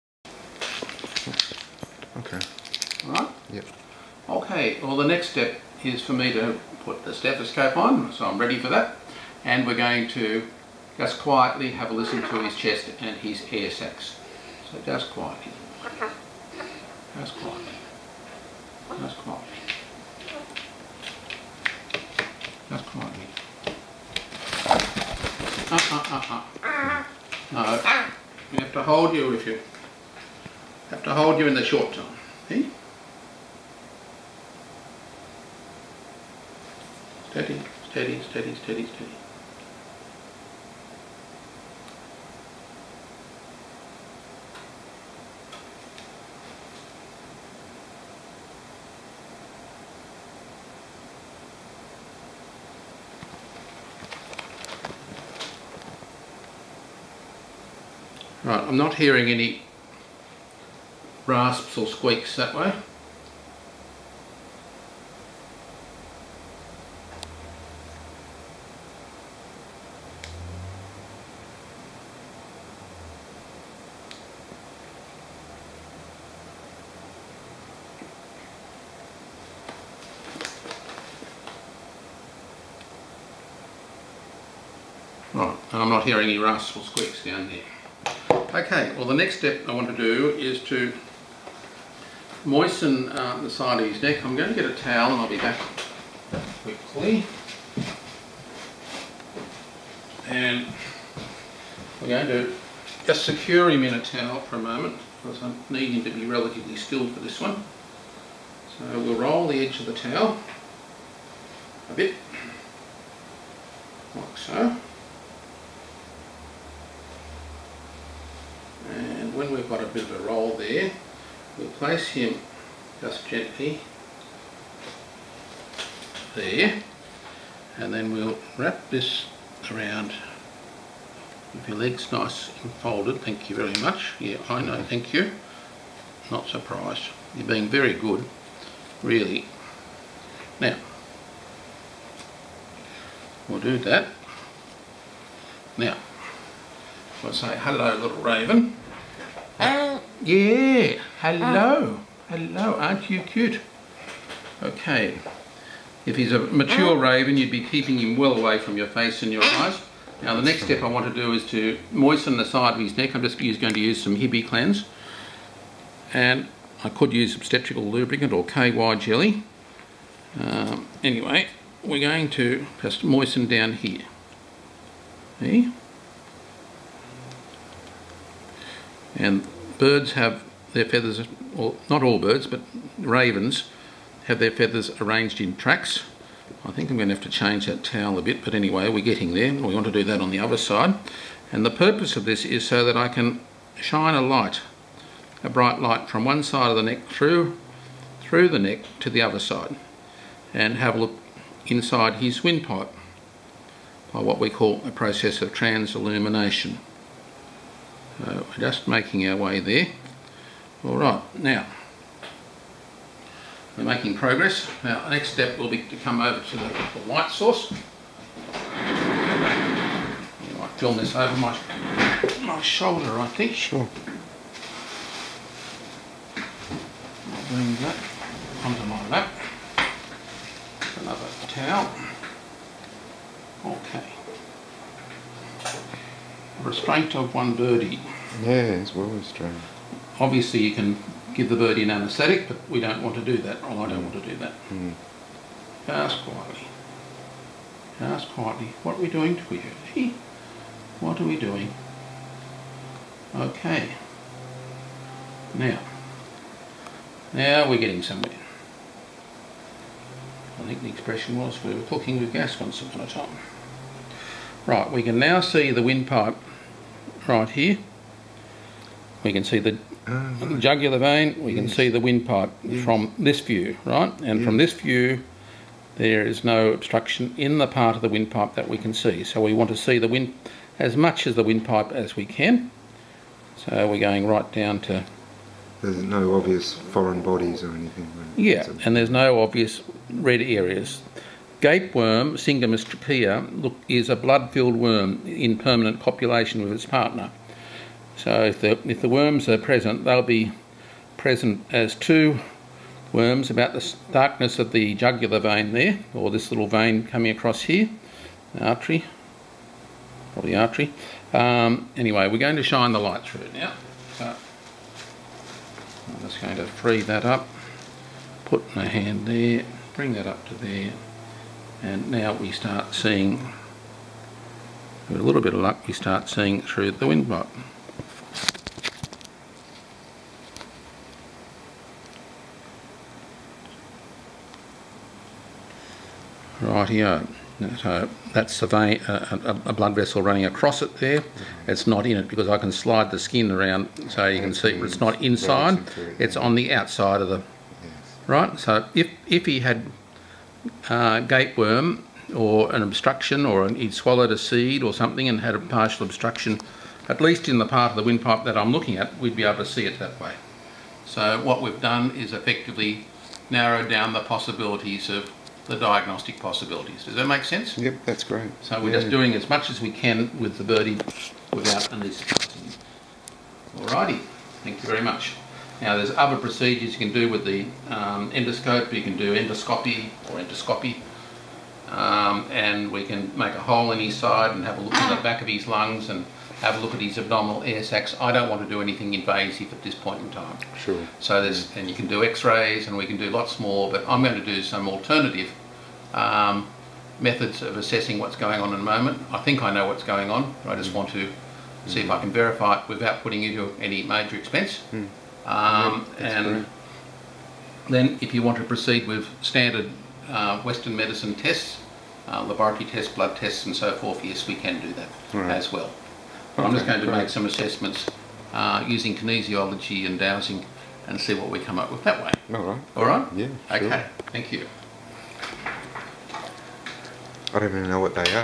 Raven Gets Short of Breath #3 131110 More Detailed Physical Exam
brings you part 1 of ?4 video segments of a consultation for an orphaned Australian Raven or Crow that was becoming short of breath after flying and had an altered voice. In this video Dr Perry starts physically examining the raven and notes the presence of many fault lines across most feathers and some other changes in the plumage that are suggestive of raven circovirus infection which he co-discovered and described with Dr Shane Raidal.
In part 2,  Dr Ross Perry discusses the potential significance of the observation that the Raven's voice is not quite normal. He suspects Aspergillosis and discusses other potential causes such as Gapeworm , Syngamus trachea, tracheal mites, chlamydia and inhaled foreign bodies.

In #3, this video, Dr Ross Perry auscultates or uses a stethoscope to listen to the lung and air sac sounds of the raven. He then parts the feathers on both sides of the raven's neck so he can see the windpipe and then see through the windpipe or trachea using a process called transtracheal illumination, a vets term for shining a light through the windpipe from one side of the neck to the other to help see more detail within the windpipe. Be sure to watch and learn from the videos that will follow in this series.